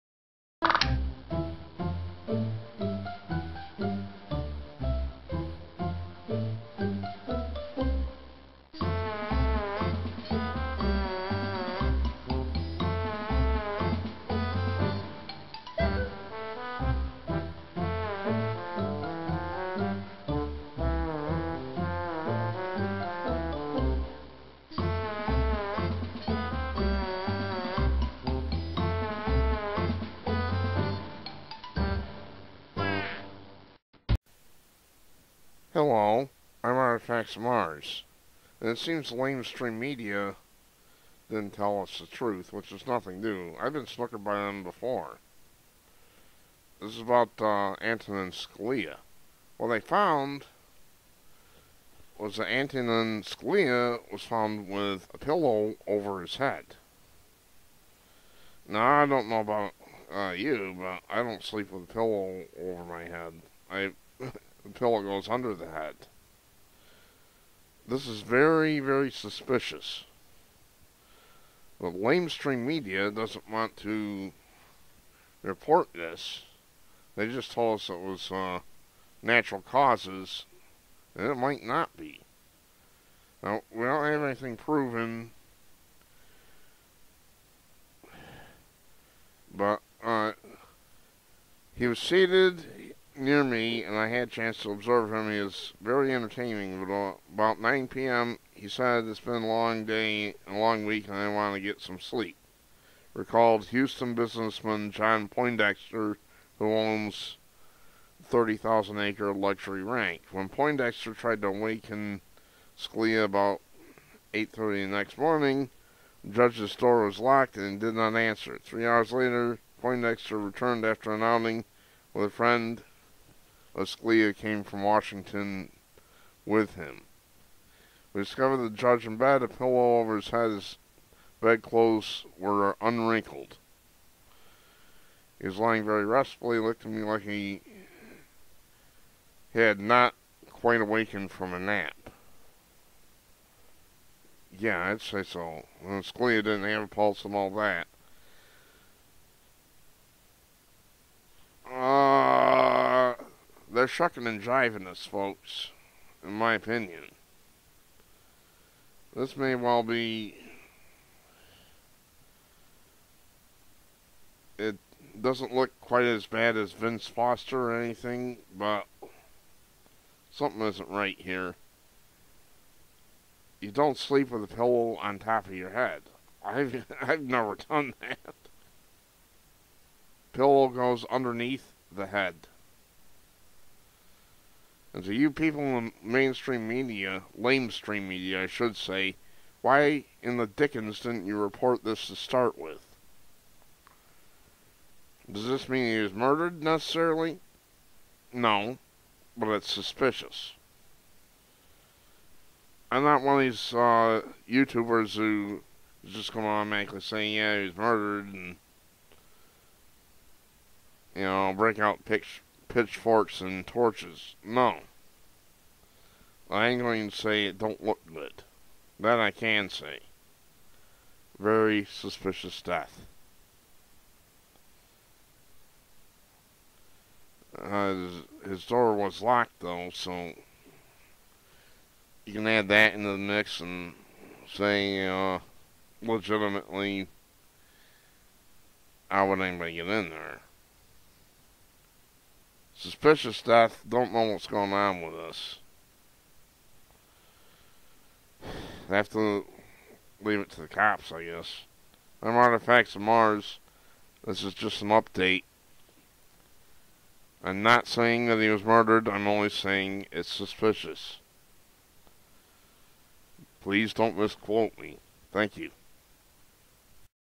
Liberal media not reporting suspicious circumstances of Scalia's death
Justice Scalia was apparently found with a pillow over his head in his room which was supposedly locked. The lame stream liberal media did not report this detail. This was not a heart attack as was reported.